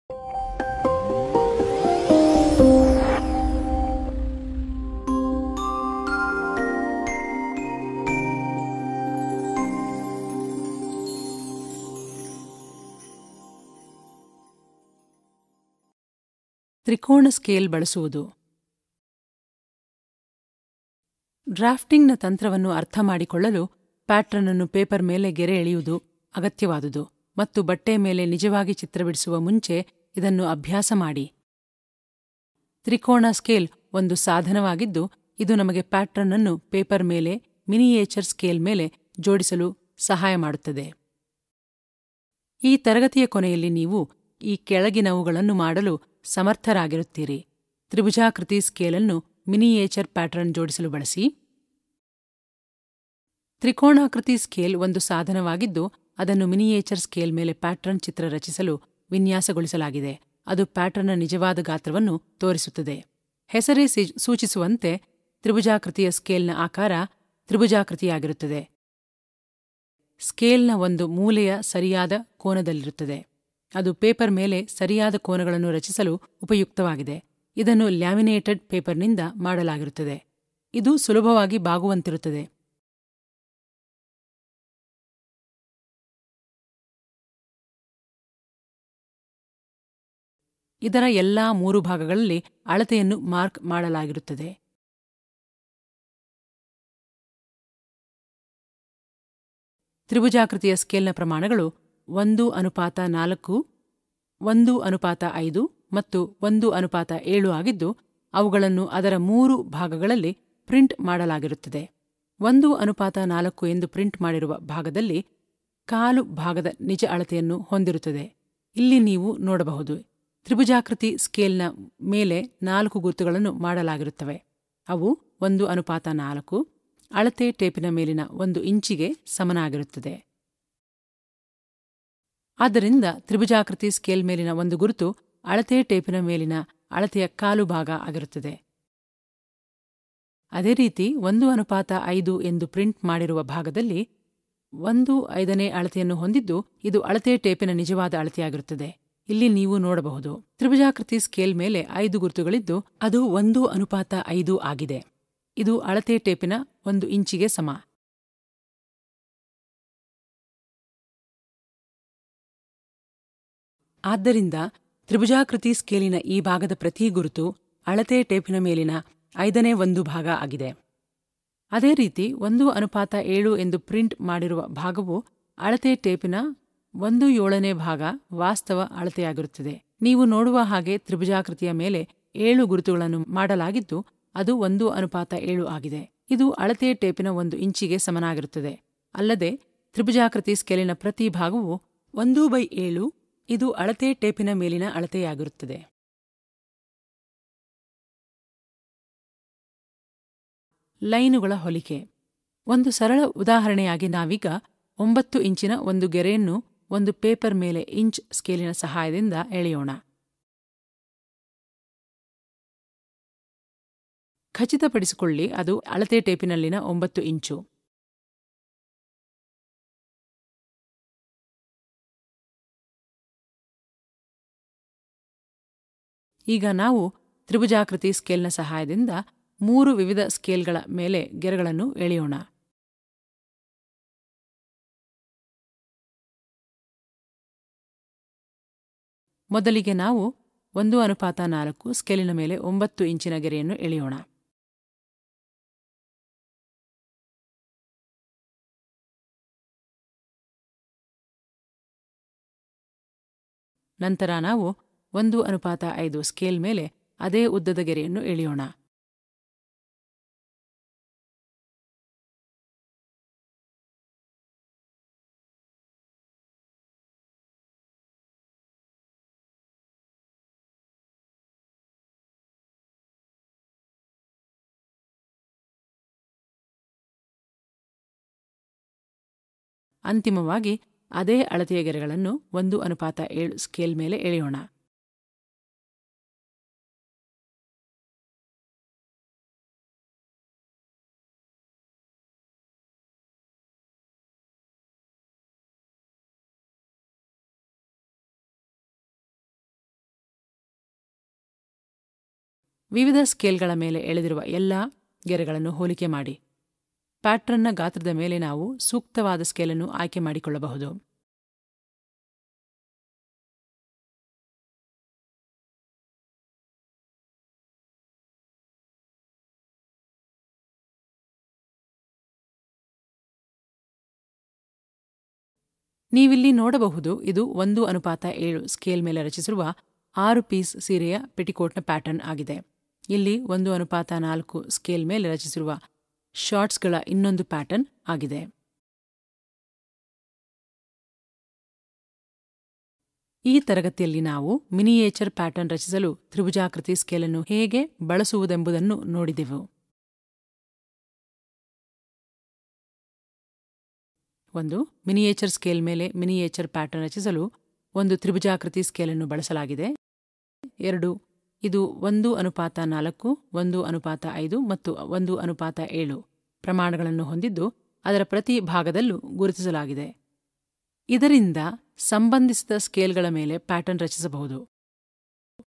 1.14.6 Use Of Triangular Scale (Kannada) (ಕನ್ನಡ)
This video explains the use of a Triangular Scale to construct miniature patterns.
Triangular Scale is a tool which will help us in constructing the patterns on paper on a miniature scale.